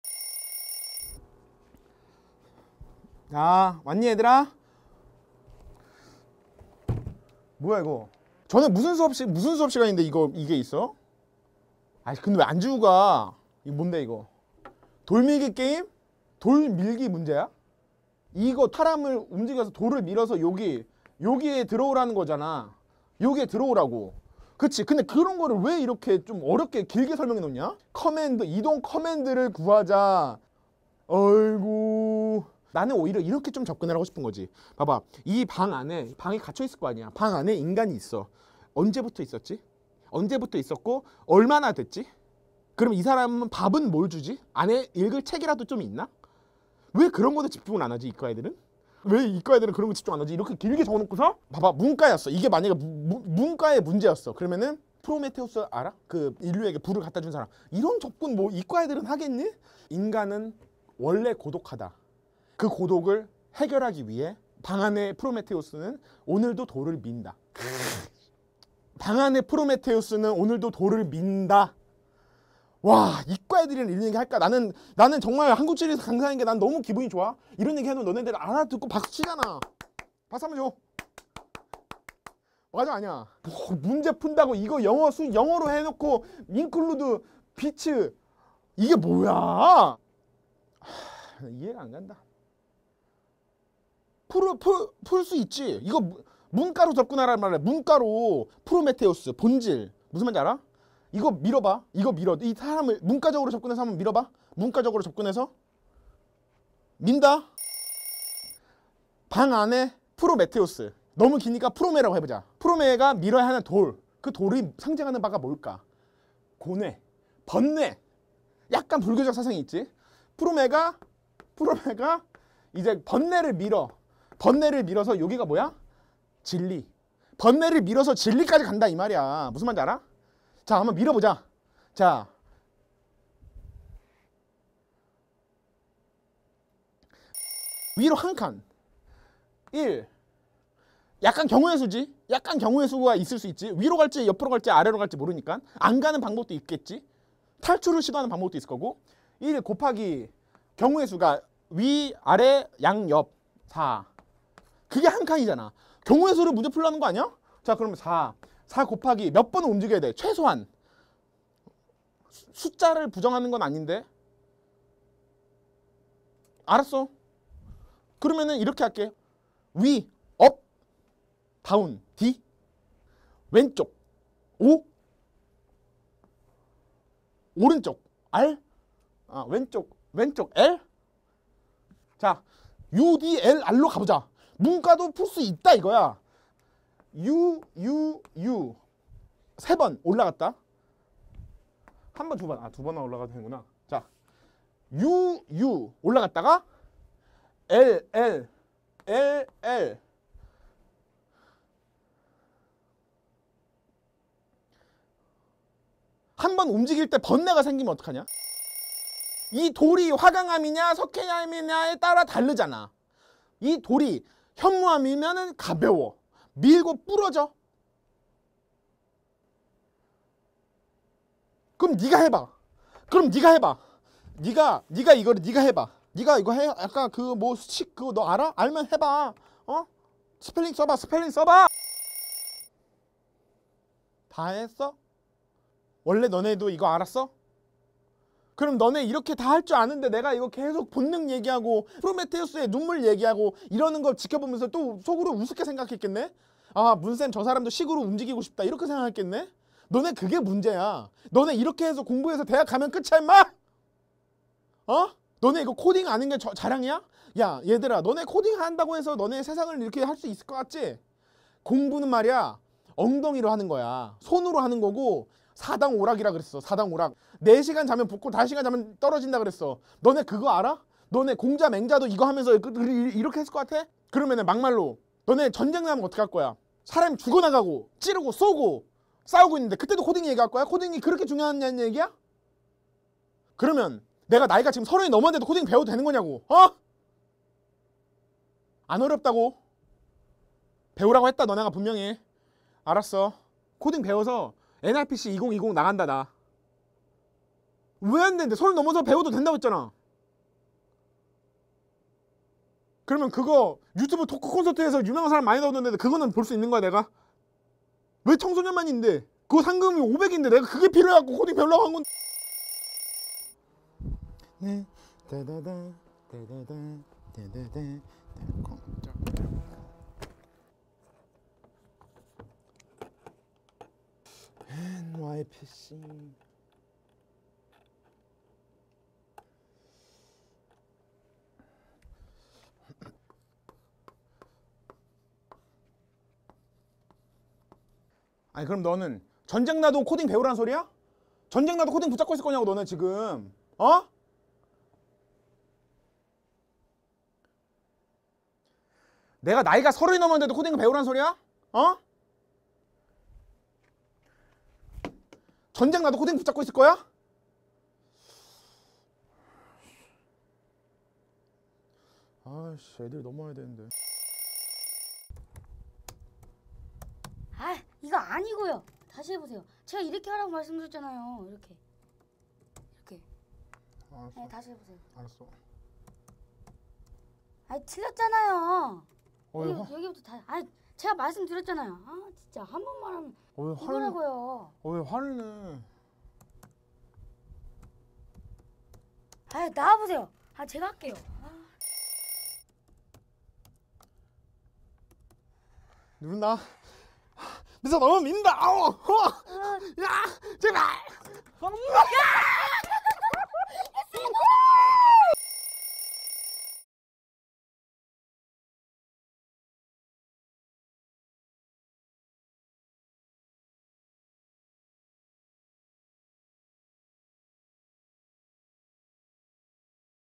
[Full ver]NYPCX빠더너스 온라인설명회
세상을 바꾸는 코딩!
제5회 넥슨 청소년 프로그래밍 챌린지 ‘NYPC 2020’ 온라인 설명회 영상이 공개되었습니다.

올해는 문쌤 컨셉으로 인기있는 크리에이터 ‘빠더너스’님이 온라인설명회를 진행해 주셨습니다!

NYPC 기출문제를 본 한국지리 1타 강사 문쌤 반응이 궁금하시다면?
지금 NYPCX빠더너스 온라인설명회 영상에서 확인해보세요!